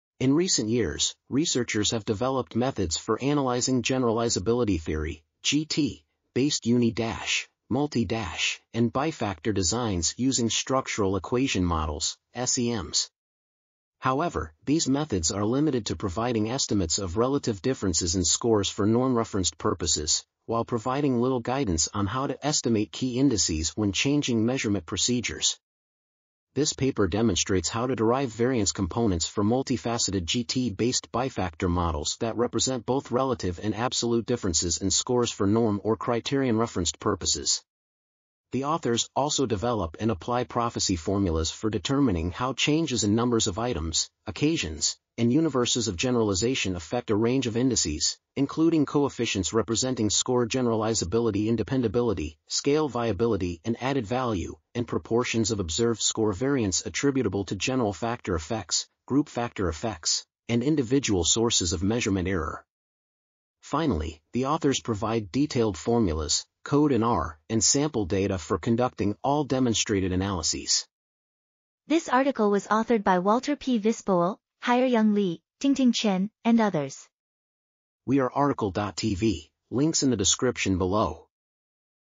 Extending Applications of Generalizability Theory-Based Bifactor Model Designs | RTCL.TV
### Keywords ###

### Article Attribution ###
Title: Extending Applications of Generalizability Theory-Based Bifactor Model Designs
Authors: Walter P. Vispoel, Hyeryung Lee, Tingting Chen ,and Hyeri Hong

### Video Timestamps ###
0:00:00 - Summary
0:01:11 - Title
0:01:17 - Outro
0:01:22 - End